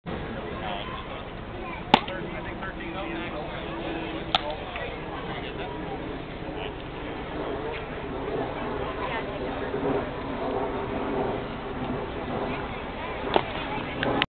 Saturday Night Grand Prairie AirHogs Baseball 8-4-2012 008
Another Saturday night at QuikTrip Park in Grand Prairie watching the Grand Prairie AirHogs.